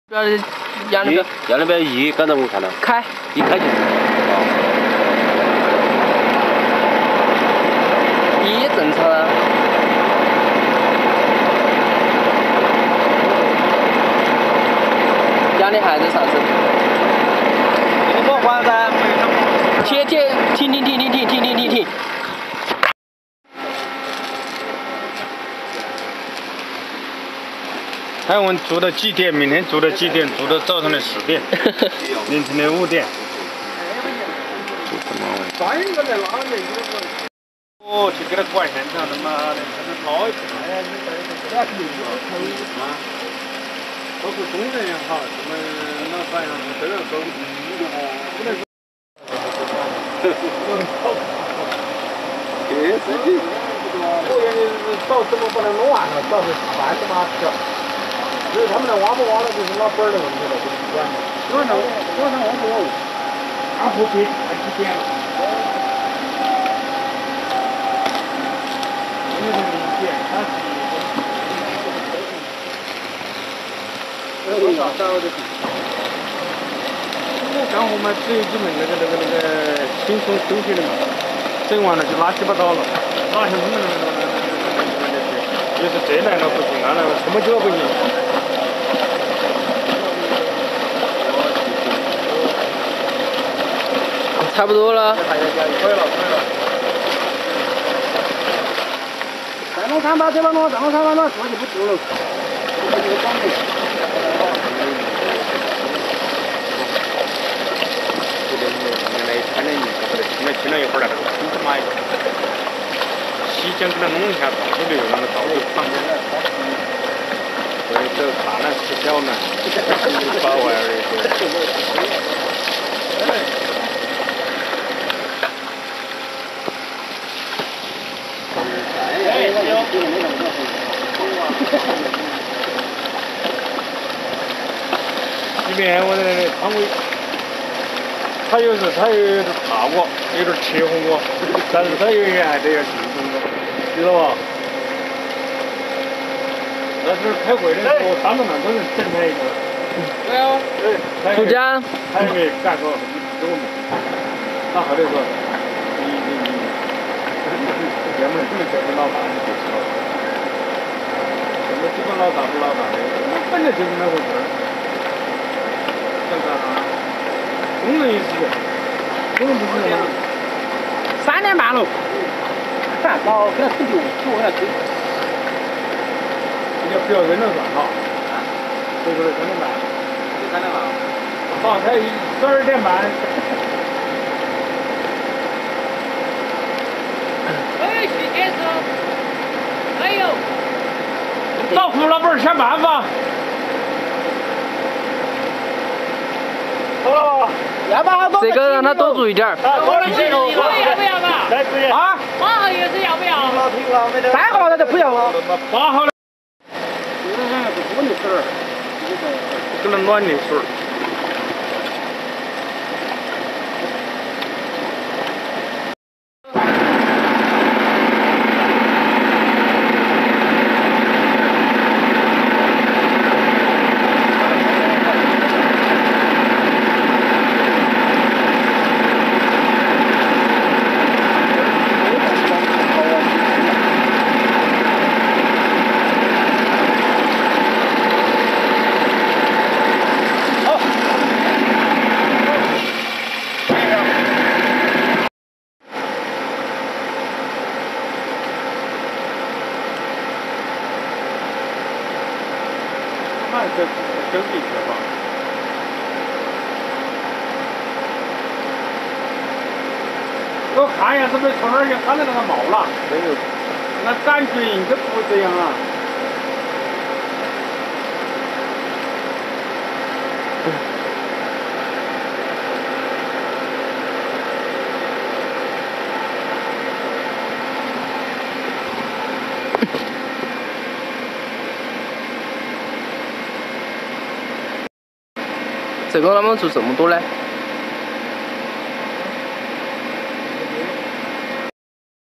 Dry Cement Mortar Spraying Machine
Working Capacity :3 Cubic meters per hour and the  you can mix the material directly in the machine .
And the water can directly connect to machine not like the worker done ,this video made in working site really work.